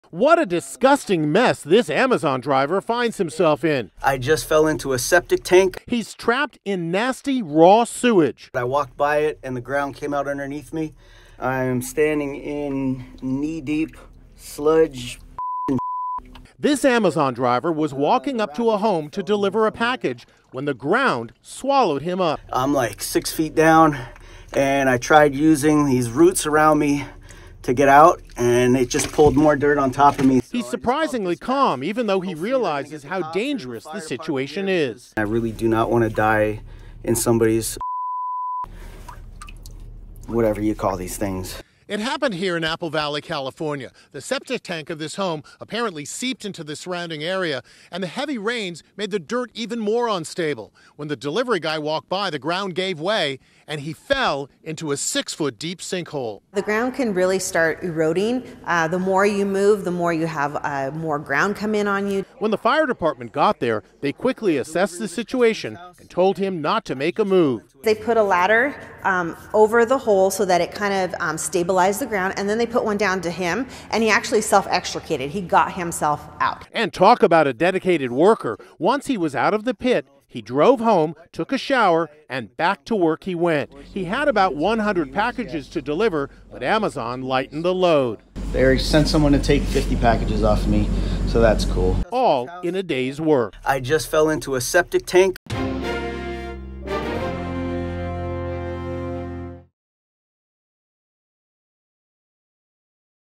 Amazon Driver Falls Into Raw Sewage, Then Goes Back to Work
An Amazon driver named Charles was walking up to a home in Apple Valley, California, to deliver a package when the ground swallowed him up. The unlucky man found himself immersed in the raw sewage of a septic tank. Charles remained calm while firefighters rescued him. Afterwards, he still had to deliver some 50 packages for Amazon. The company is owned by Jeff Bezos, one of the richest men in the world.